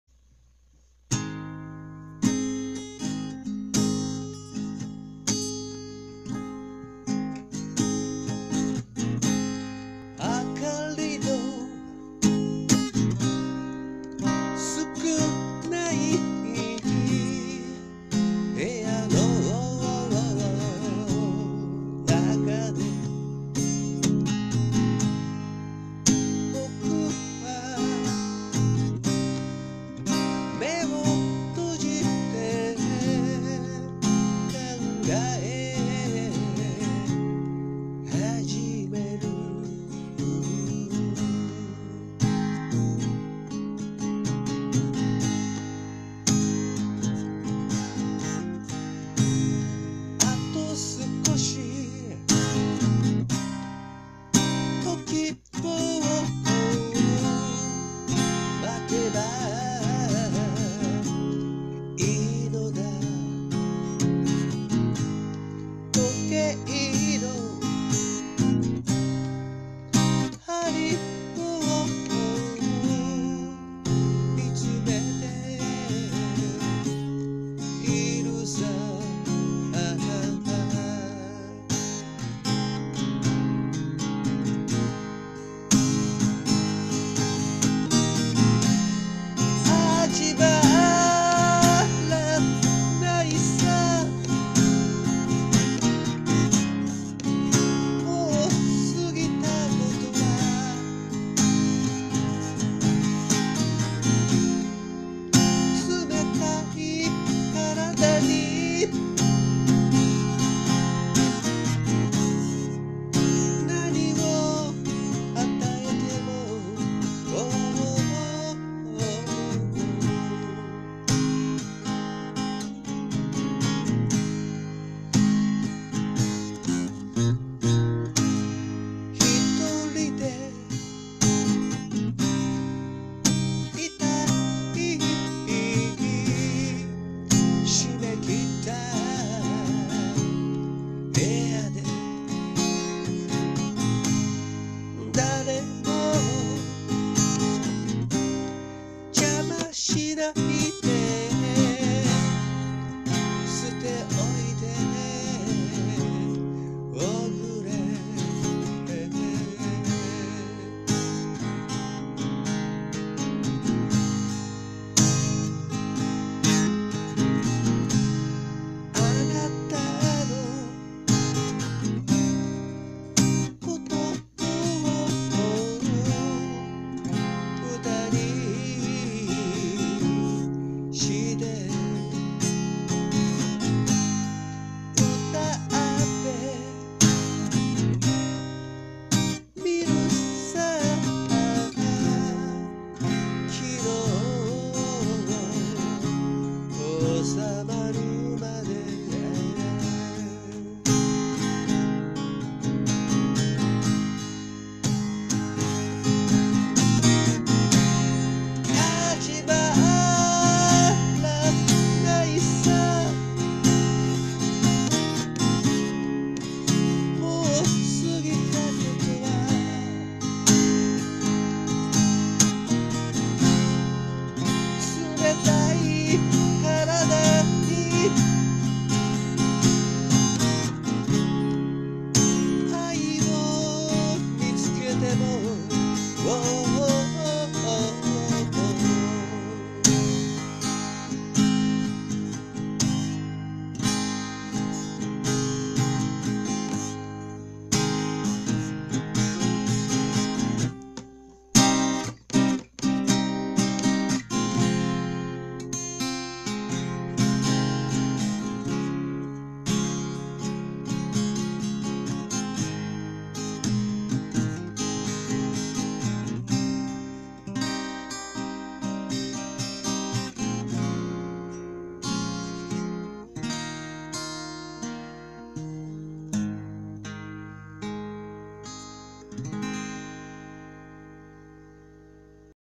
by NESO YAMAHAポプコン入選曲（カバー）曲名不明
部屋片付けてたらカセットテープ出てきて
この曲も入ってた・・・。曲名わからないです。
当時お気に入りでよく口ずさんでいました。